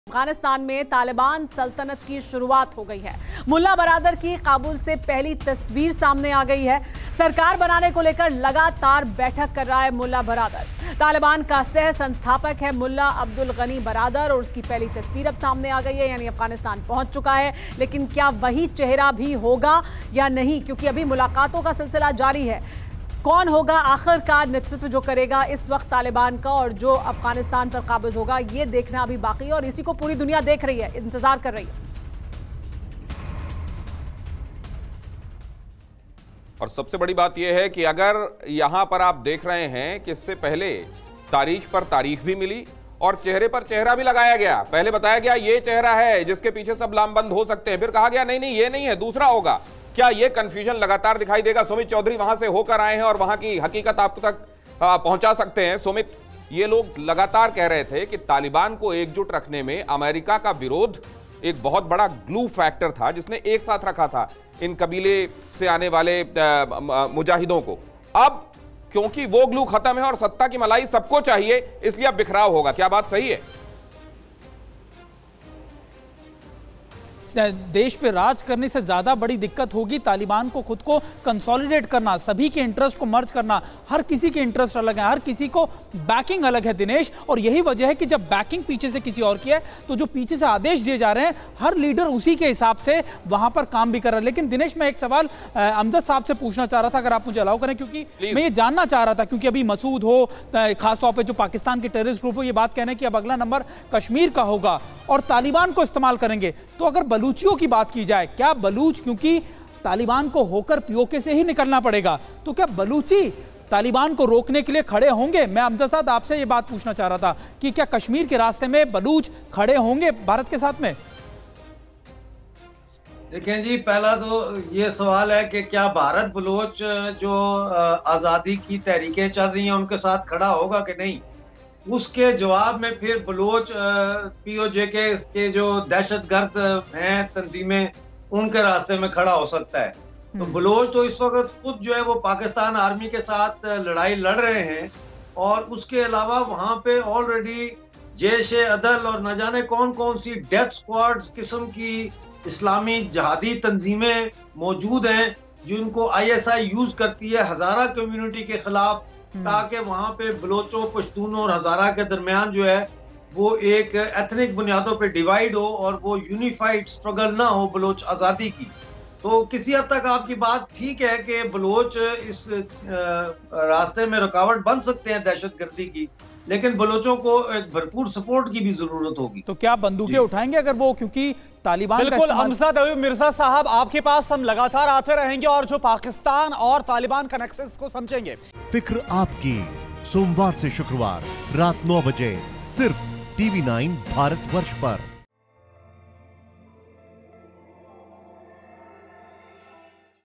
सरकार बनाने को लेकर लगातार बैठक कर रहा Mullah Abdul Ghani Baradar, पहली तस्वीर आई सामने
अफगानिस्तान में तालिबान सल्तनत की शुरूआत हो गई है. मुल्ला बरादर की काबुल से पहली तस्वीर सामने आई है. सरकार बनाने को लेकर बरादर लगातार बैठक कर रहा है. मुल्ला अब्दुल गनी बरादर (Mullah Abdul Ghani Baradar) तालिबान का सह-संस्थापक है.

Credit